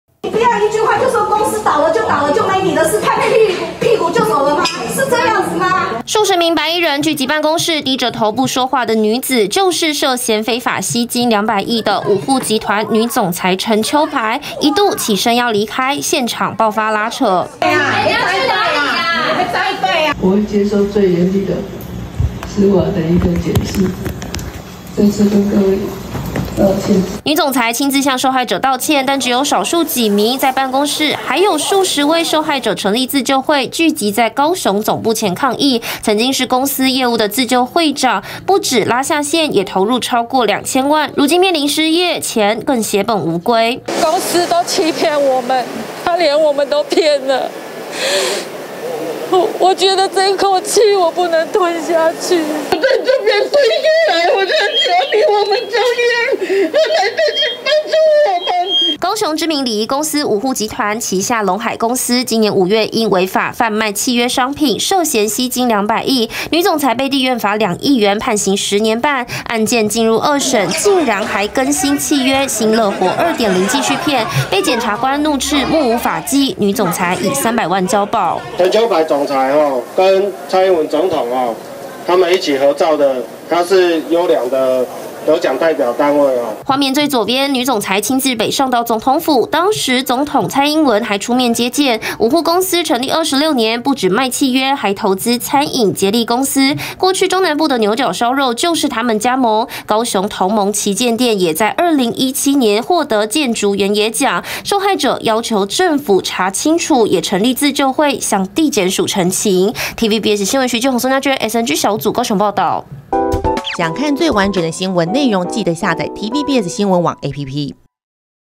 五互集團涉非法吸金220億! 女總裁現身道歉 遭圍剿爆衝突｜TVBS新聞
牛角燒肉的加盟店「捷利餐飲集團」，因為母公司「五互集團」涉嫌非法吸金220億，上個月無預警歇業。而五互集團旗下「龍海事業」違法販售契約商品，全台也有兩萬多名受害者，決定組成自救會，前往高雄總公司陳情，總裁陳秋白也現身辦公室，場面火爆，一度爆發拉扯。